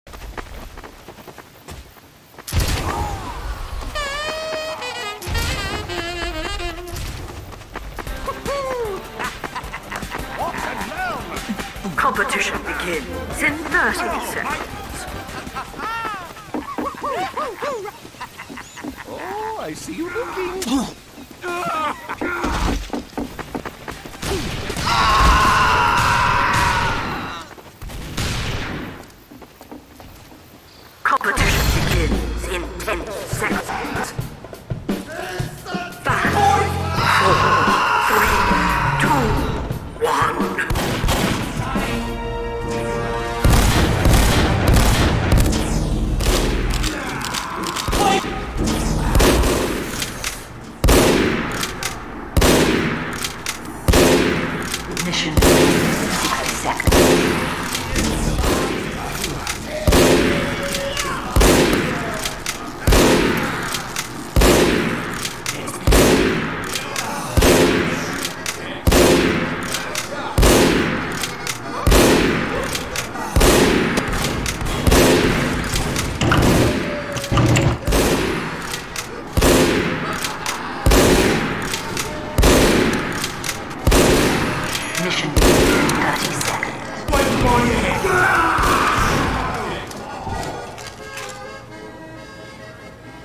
Sniper Duel of the Century
I AM THE KING OF AUSTRALIA !!!

0:00 - T-Posing as scout
0:41 - Sniper Duel